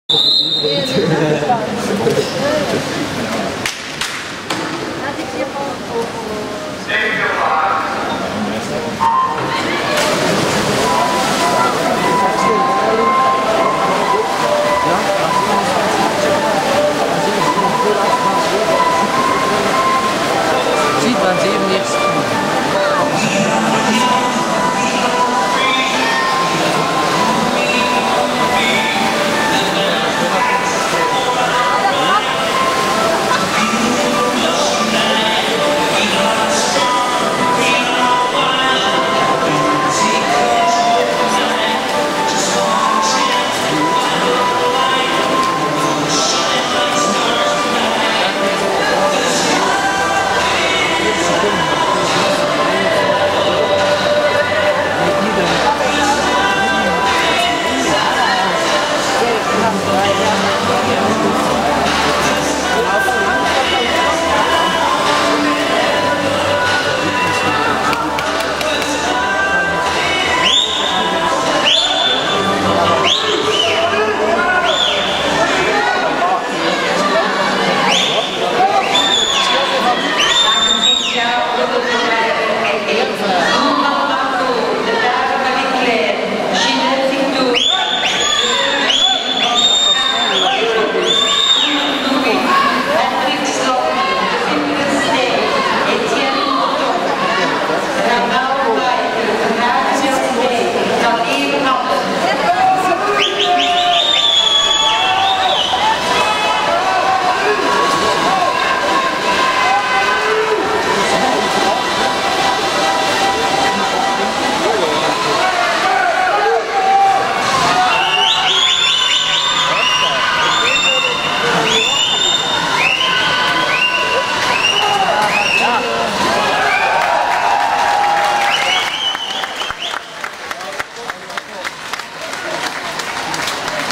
BK Gent ‘13 - 200 wissel (finale) - Ensger
Ensger brons in baan 5 op Belgische Kampioenschappen.

2:02.04 (PR)